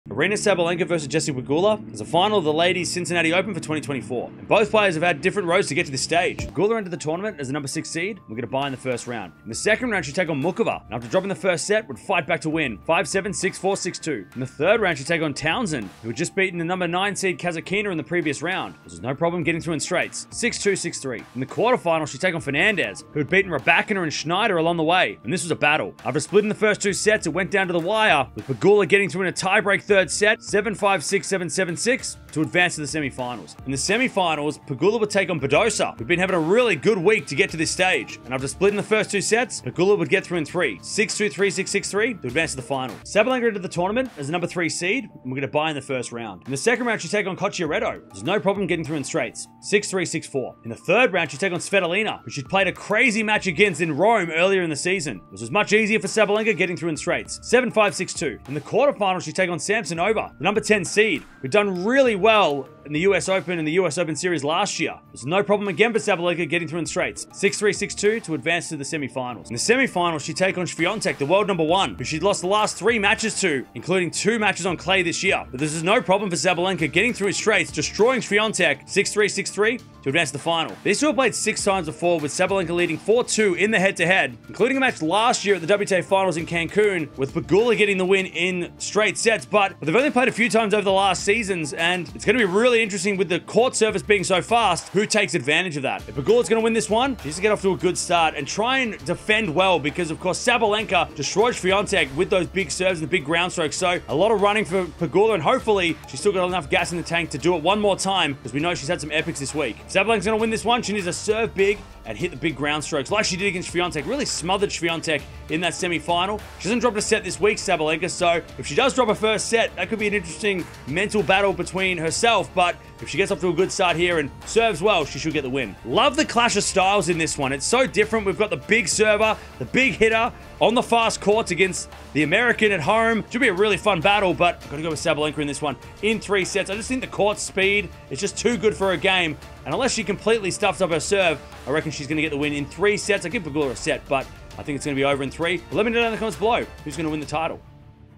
Sabalenka vs Pegula | WTA Cincinnati 2024 Final | Tennis Preview & Prediction
Aryna Sabalenka will take on Jessica Pegula in the final match of the Cincinnati Open for 2024. Sabalenka just destroyed Swiatek while Pegula just faught past Fernandez and Badosa. Who will win the title?

0:00 | Intro
0:06 | Jessica Pegula
0:42 | Aryna Sabalenka
1:20 | Head to Head
1:35 | Keys to the Match
2:05 | Prediction